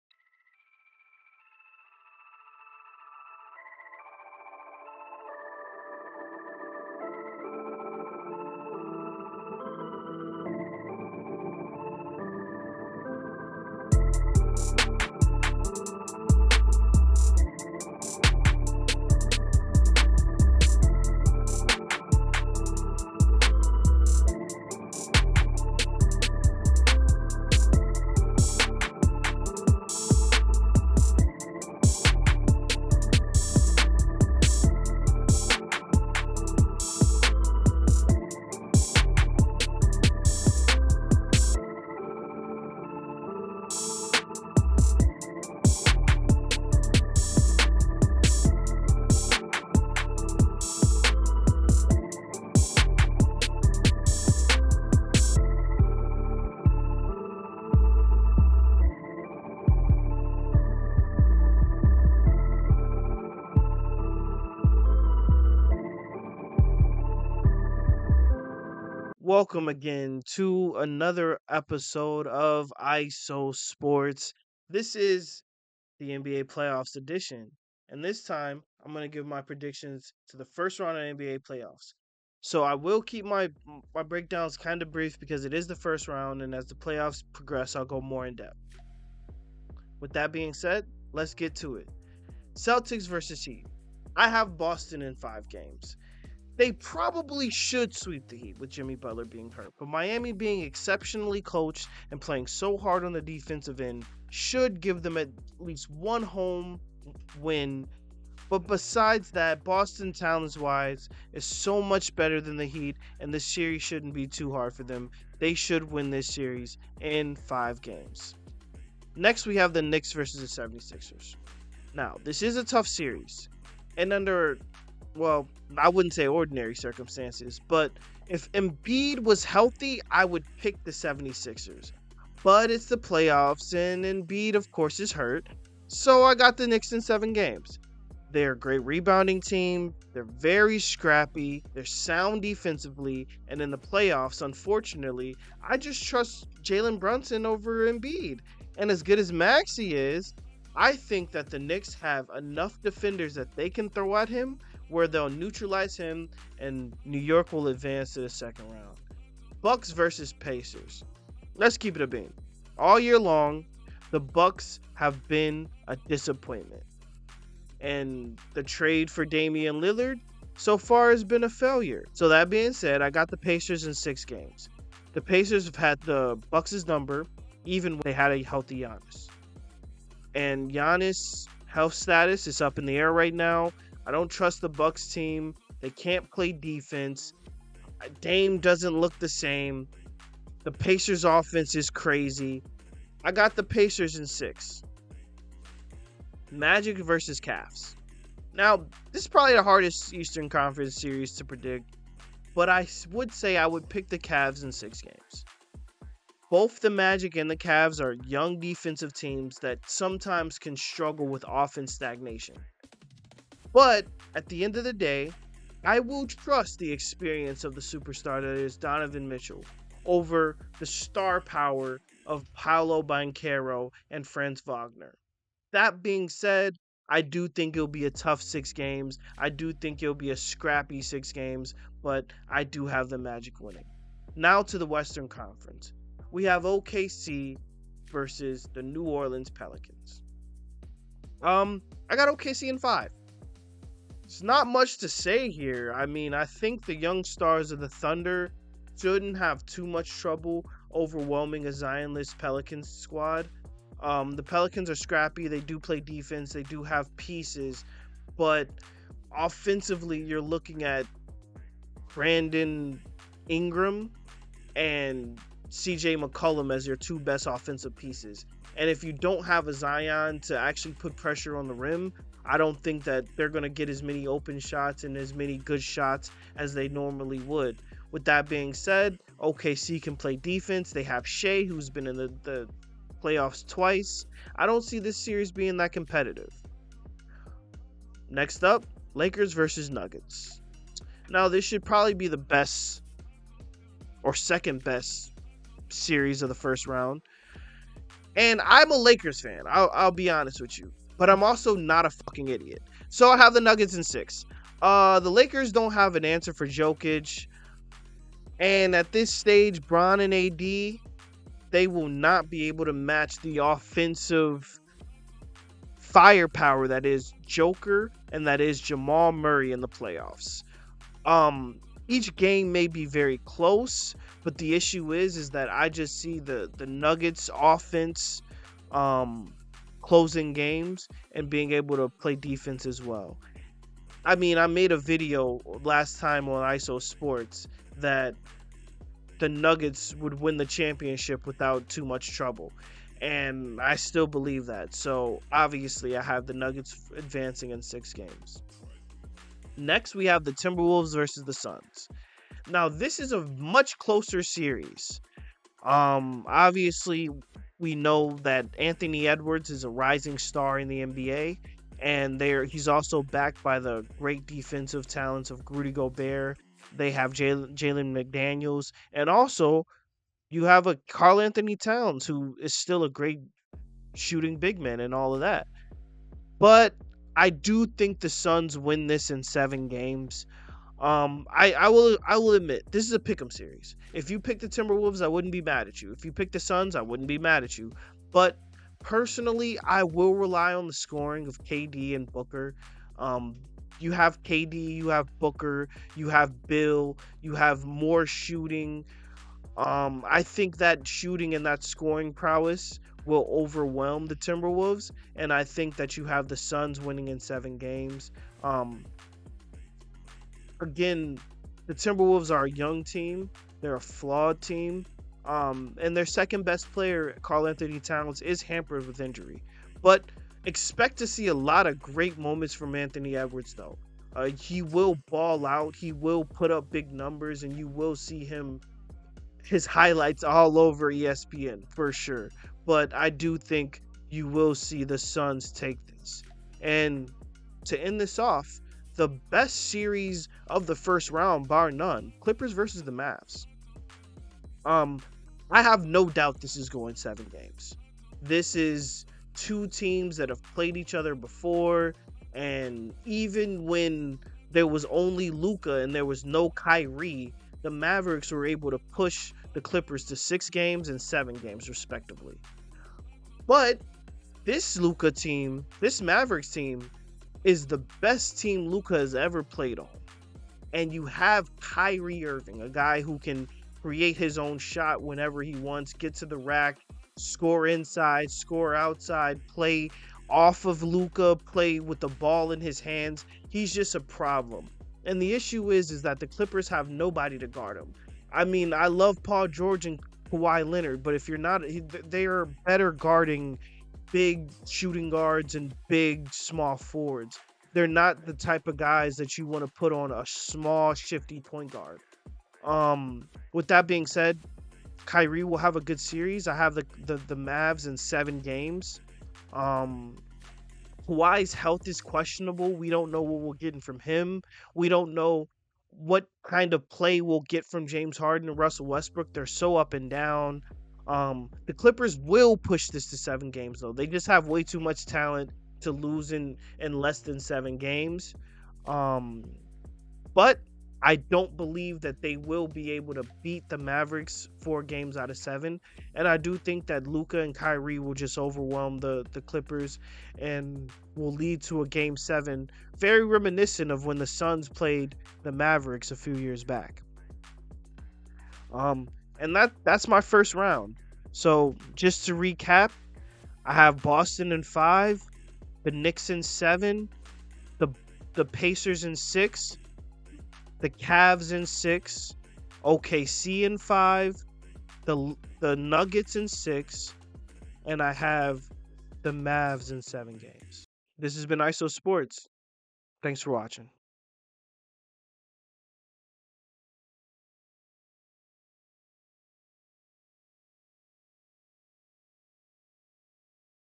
NBA Playoffs First Round Predictions | IsoSports Episode 6
Isosports is back with an NBA Playoffs edition. In this episode, Iso breaks down the first round of the playoffs and give his predictions for each series. Thanks for tuning in to your weekly dose of IsoSports.